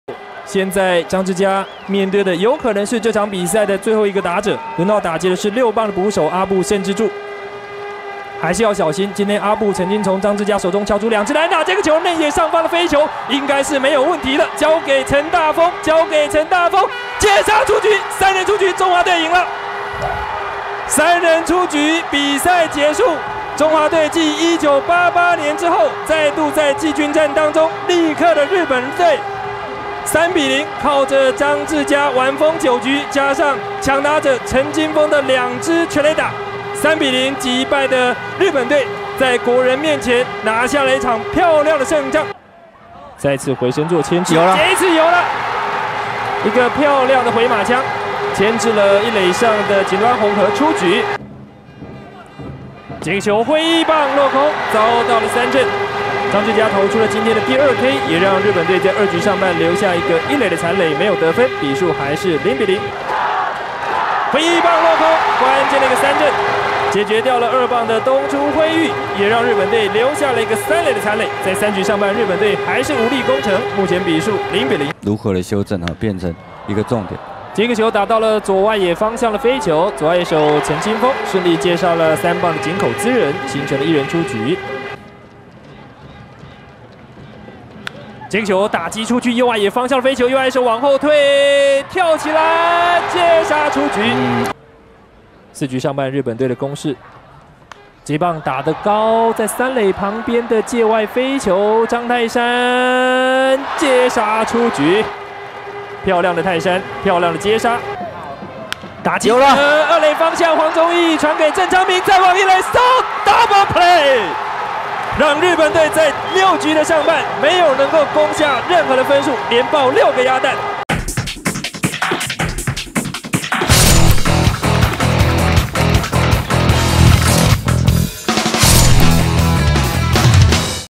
經典回顧／殞落的天才－張誌家 難忘2001年世界盃的感動｜三立娛樂星聞
畫面來源：緯來體育台授權